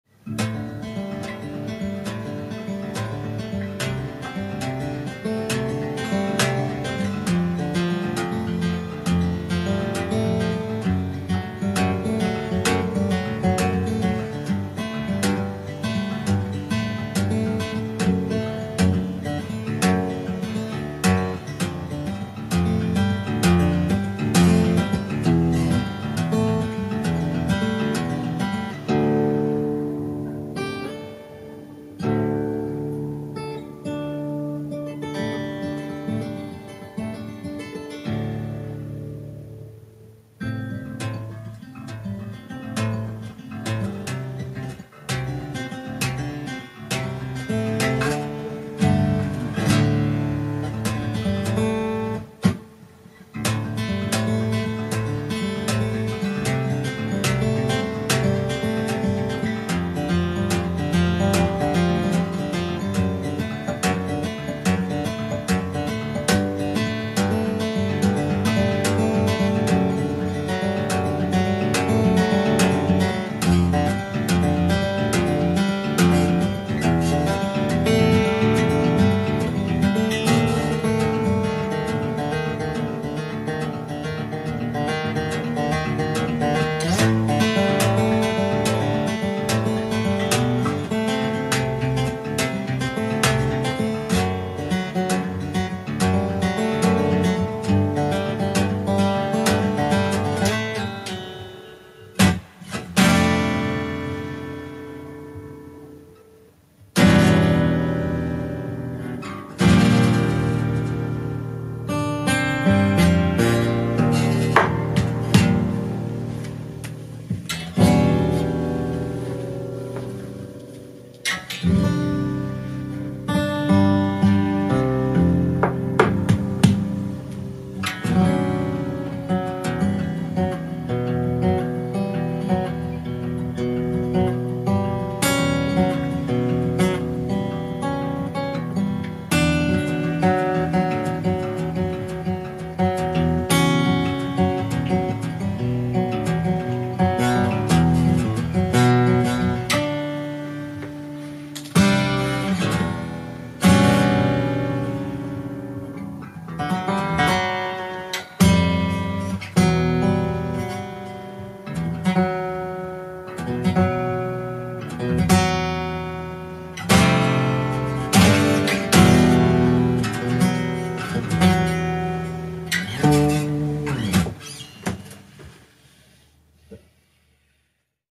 Exotic Acoustic Jam in DADGAD fingerstyle
Not without mistakes, all improvised and in the moment. Was trying out a setting with my camera and thought I capture one of my nightly jams again :)

The guitar is made by Alvarez and tuned to DADGAD.